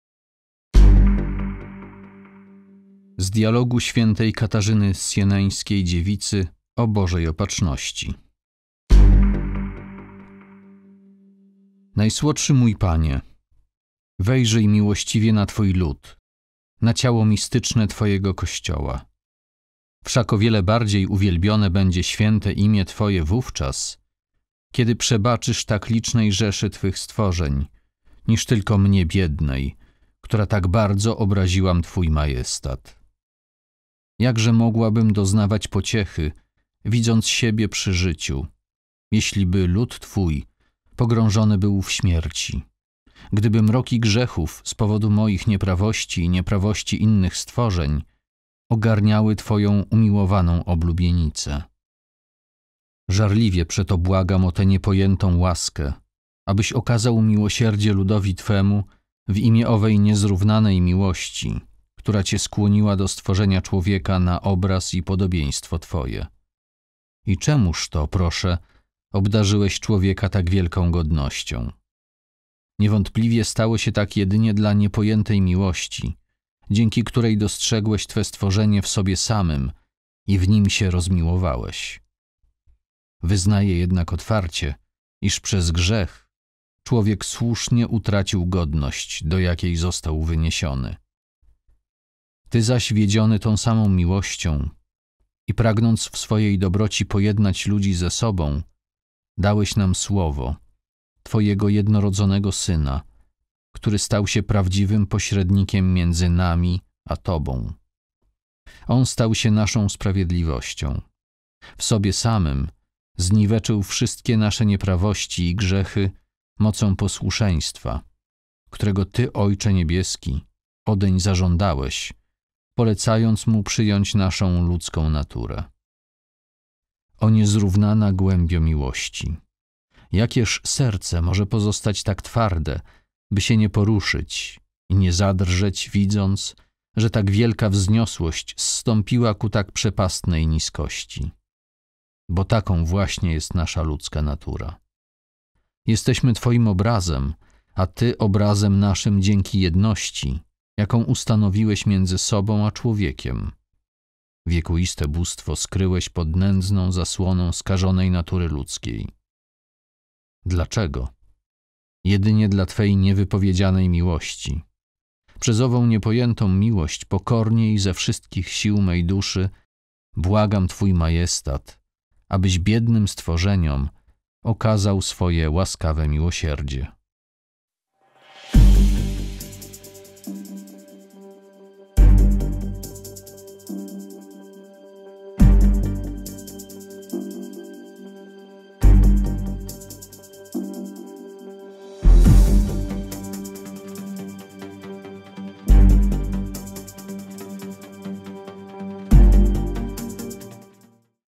GodzinaCzytań | II Czytanie | 8 sierpnia 2021
8 sierpnia 2021
XIX Niedziela zwykła, rok B

Godzina Czytań, II Czytanie
Z Dialogu św. Katarzyny Sieneńskiej, dziewicy, O Bożej Opatrzności
4, 13

Możesz też wpłacić darowiznę na konto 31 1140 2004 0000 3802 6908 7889 z dopiskiem „Darowizna Maskacjusz TV”.